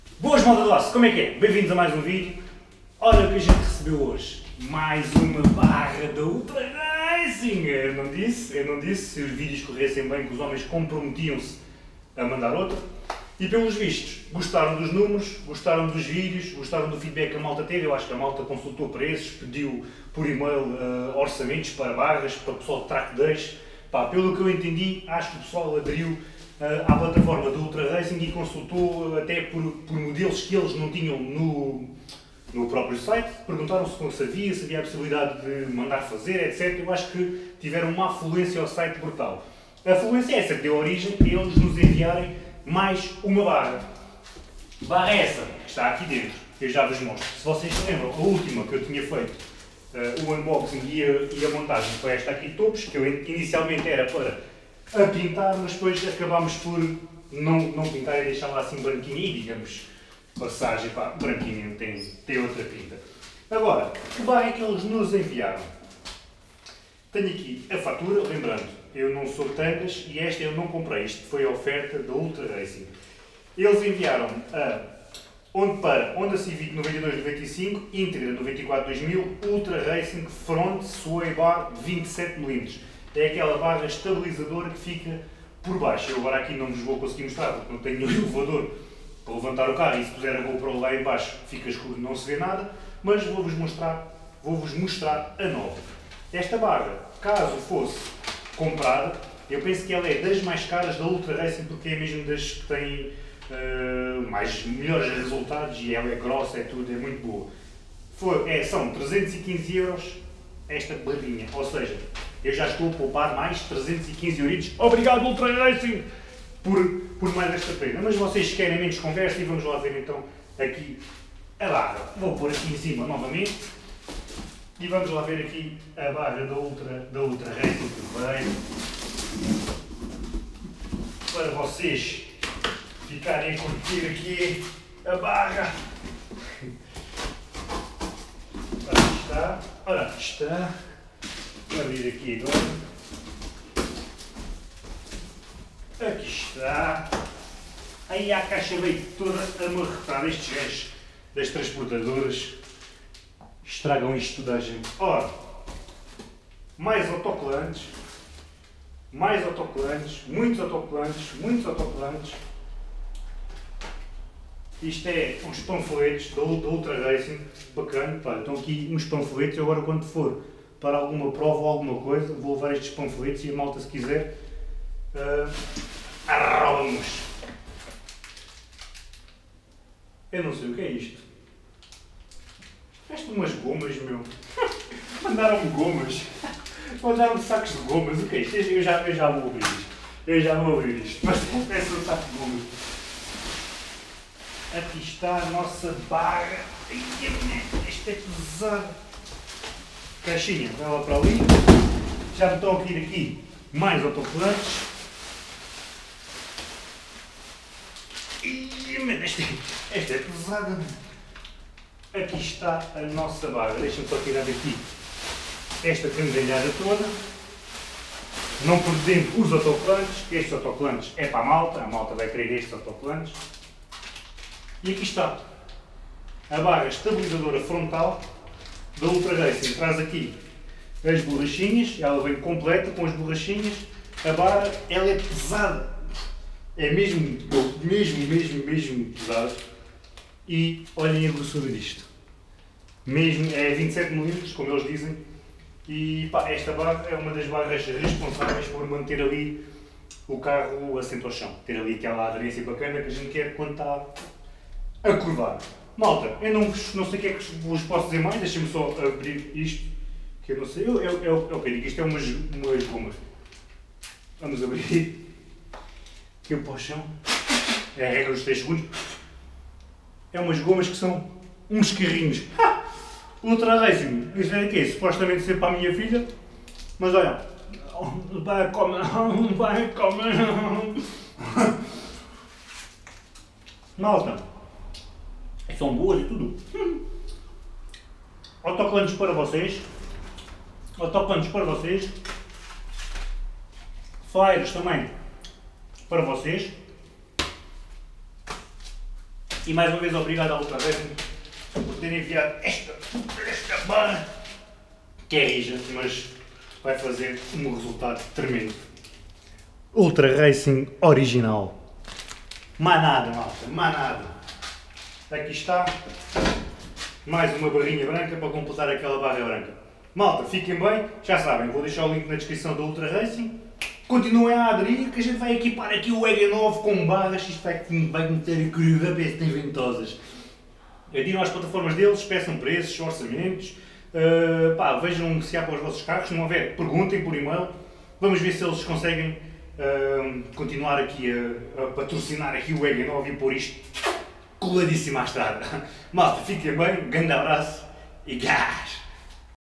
A ULTRA RACING enviou MAIS UMA BARRA - QUE TOPPPPP
Vou sortear um PUNTO GT TURBO carregado de MATERIAL

"" Concurso nº71/2018 Autorizado pela Secretaria Geral do MAI - Prémio não convertível em dinheiro ""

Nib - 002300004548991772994